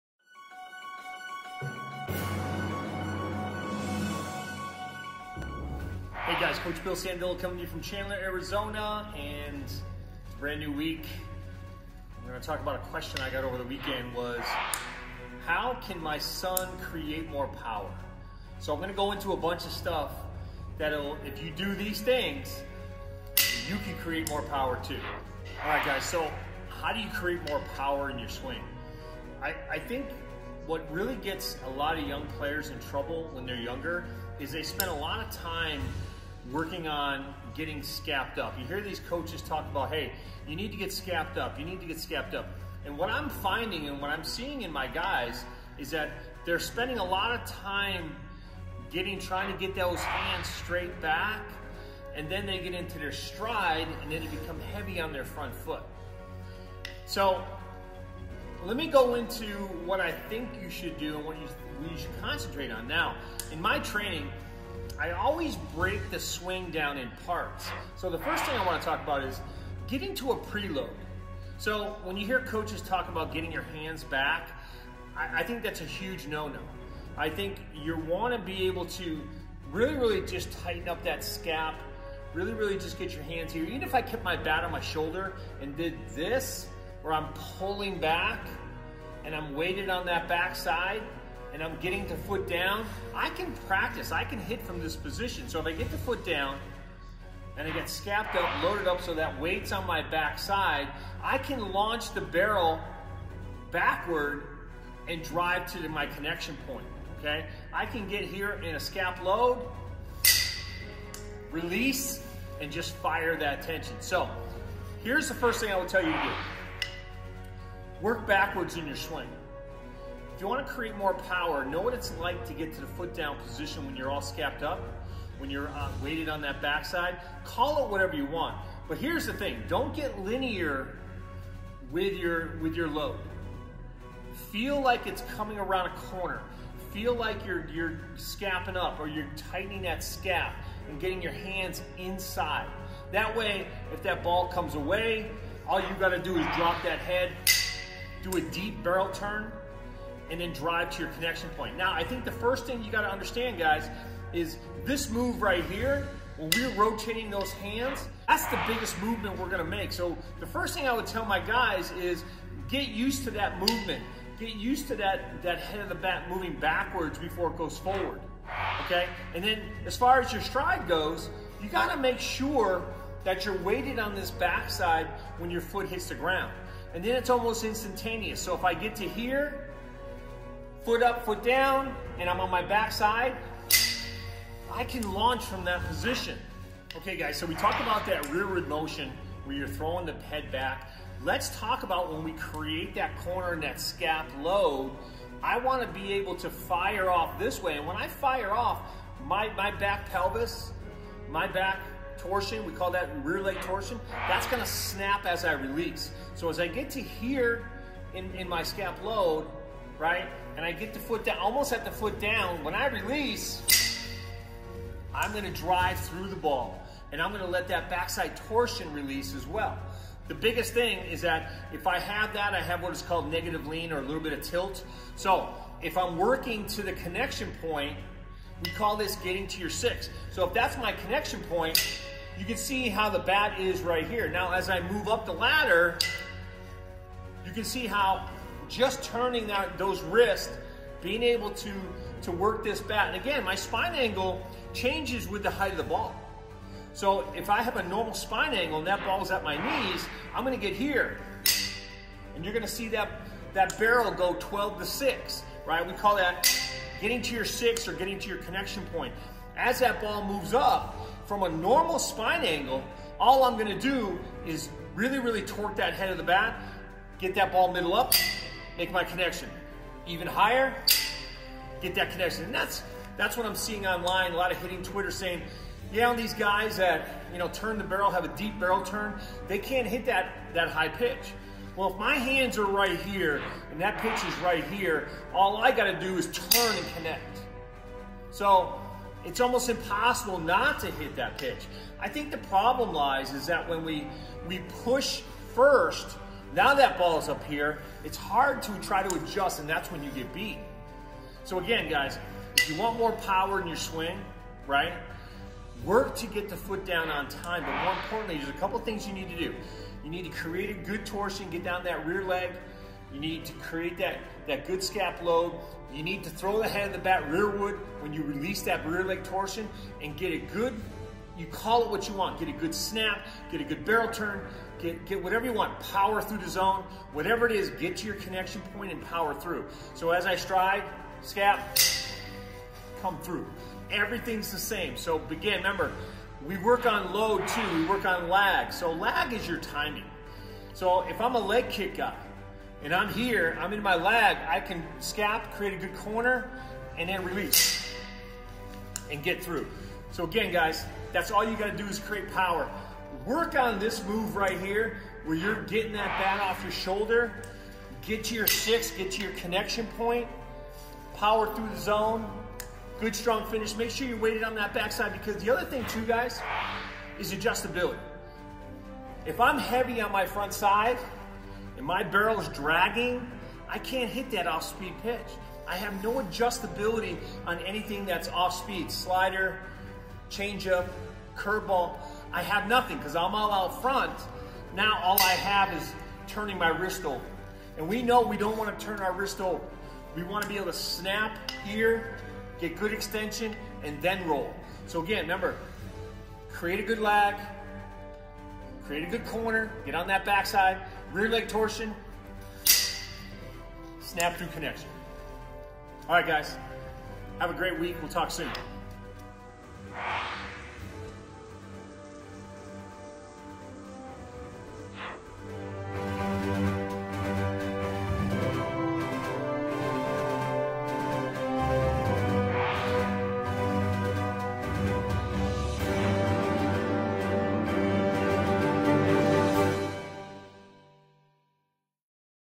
Do this to create more power in your swing
In this video we’re gonna talk about different things to create more power in your swing from understanding how your swing works to teaching it backwards to as a player really understanding what it really feels and how your swing functions to the best of your ability. We also want to talk about how to create more power by doing simple moves. Making sure your scabbing up correctly creating a good corner and then getting your foot down on time and by releasing your rear leg torsion and launching the barrel to connection